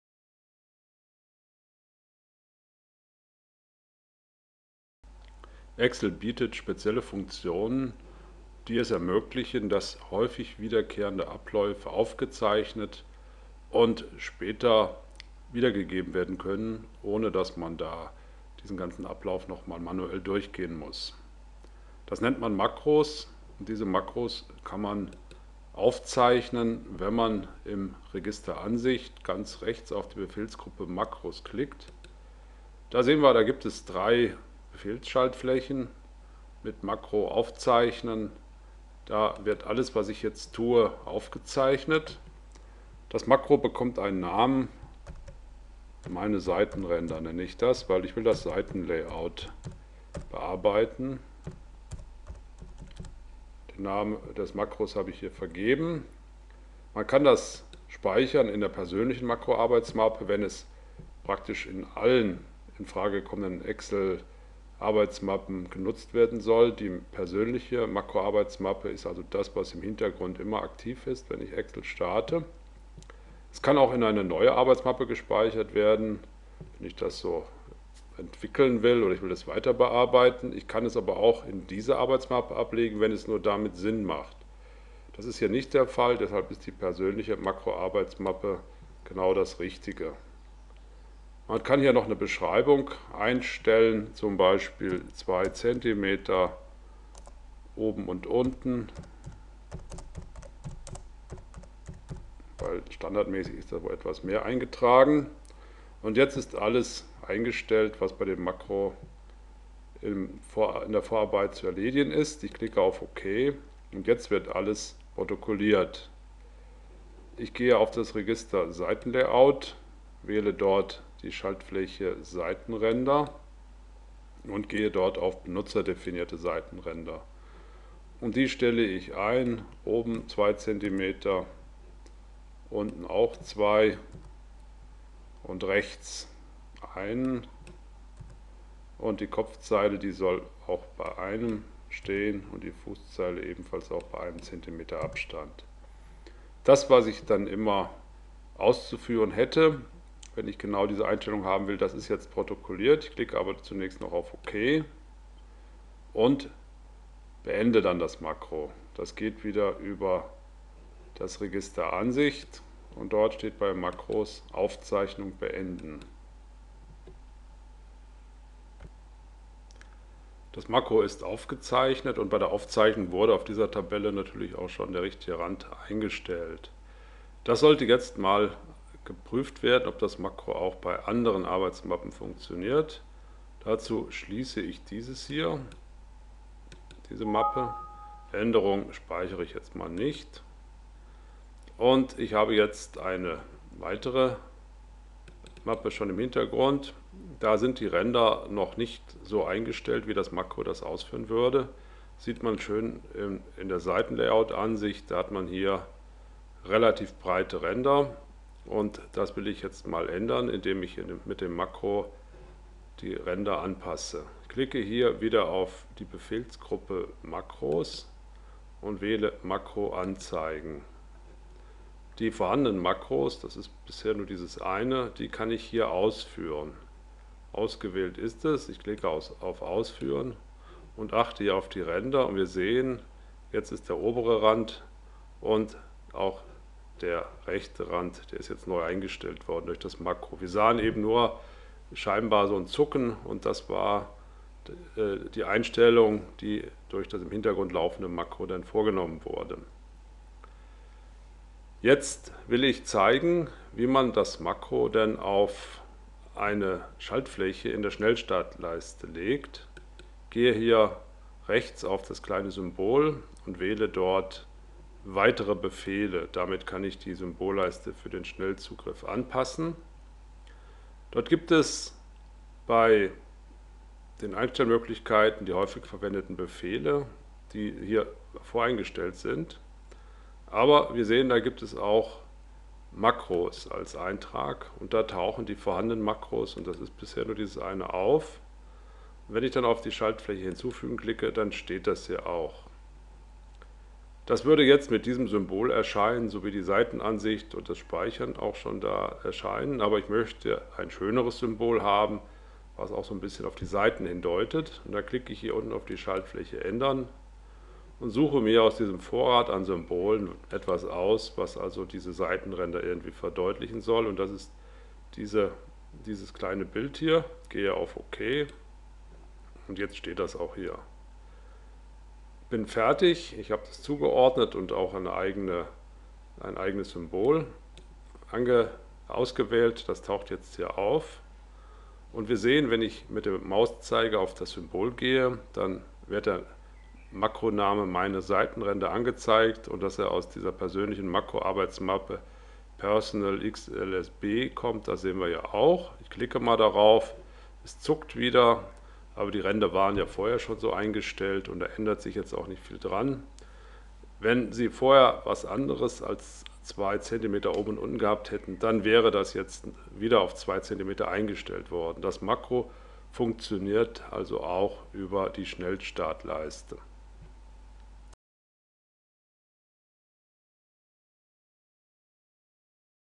am4510 - Einfache Excel-Makros erstellen
kann man den vollständigen Excel-Kurs kostenlos besuchen (Username a Password a). Hier sehen Sie Schritt für Schritt, wie ein einfaches Makro erstellt wird und man es in die Symbolleiste für den Schnellzugriff einbaut.